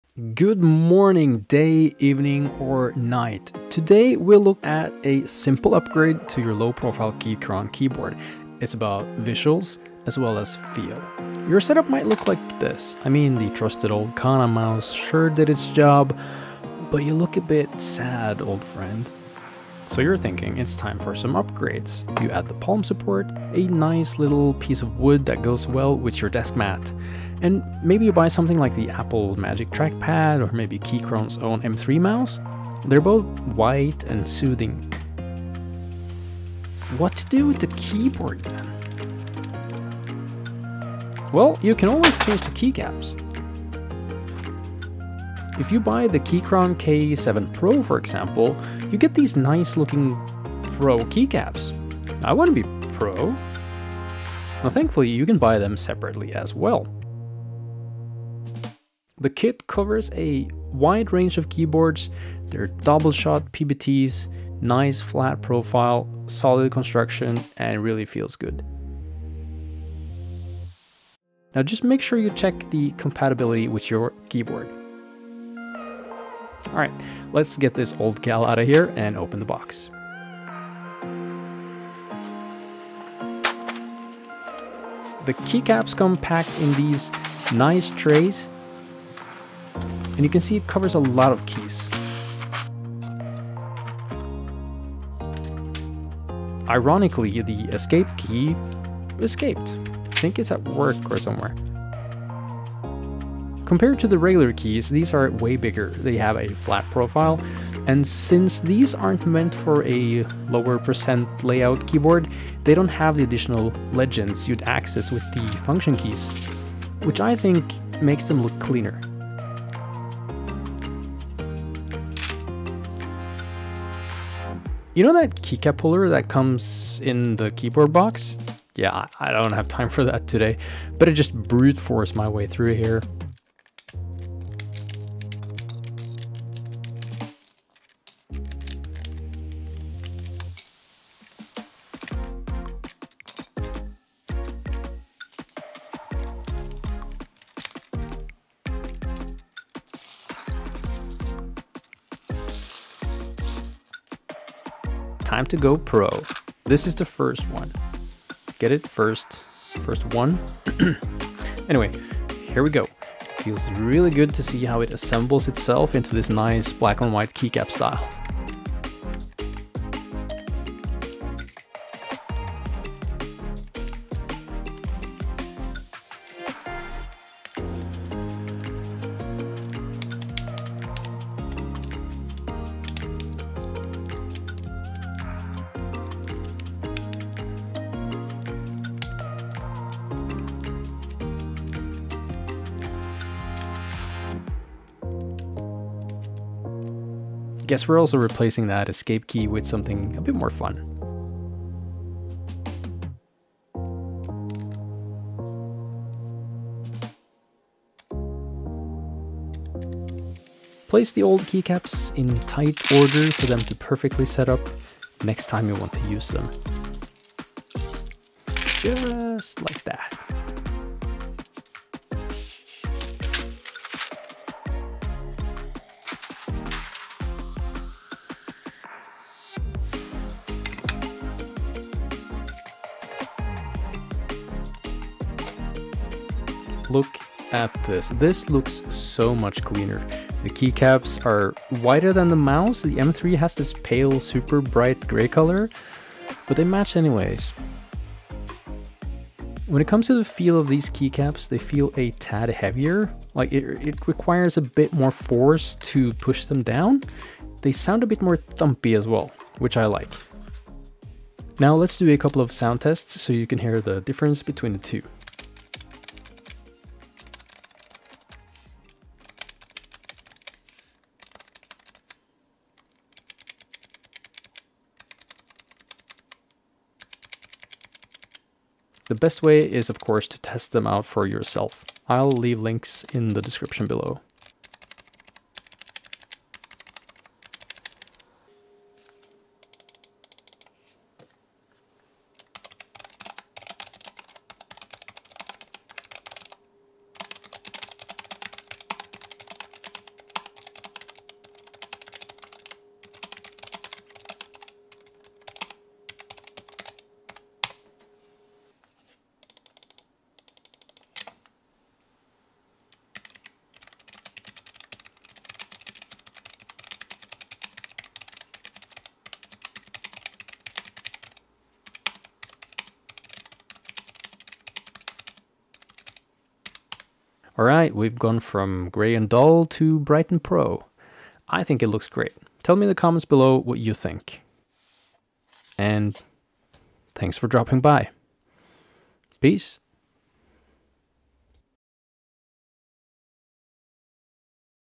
Upgrade Your Keychron Low-Profile K1/K3/K5/K7 With New Keycaps
Let's upgrade the dull and grey default low-profile Keychron keyboard of yours.

⌨️ Keychron K1 Pro:

⌨️ Keychron K3 Pro:

⌨️ Keychron K5 Pro:

⌨️ Keychron K7 Pro:

Apple Magic Trackpad:

Keychron M3 Mouse:

Looks like Amazon is out of white M3 mice.